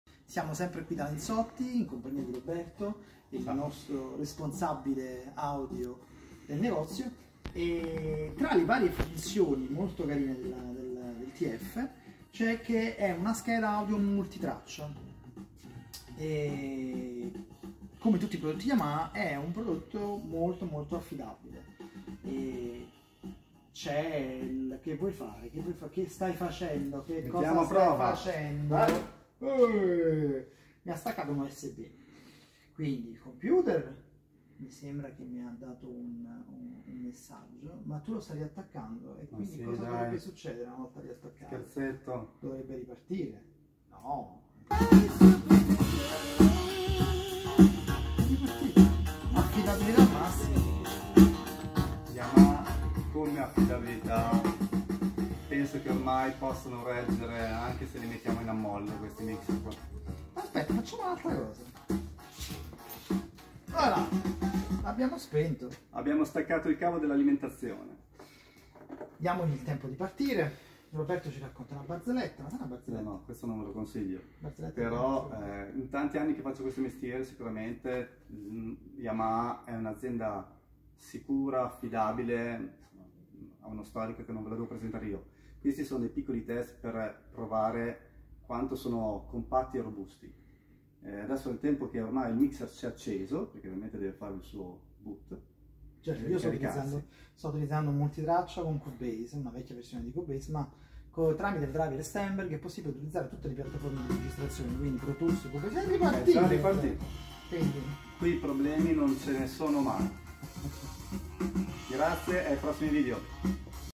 puntata9, Yamaha TF1 registra i live in multitraccia
Un mixer che registra i nostri concerti in multitraccia ma con tutta la sicurezza Yamaha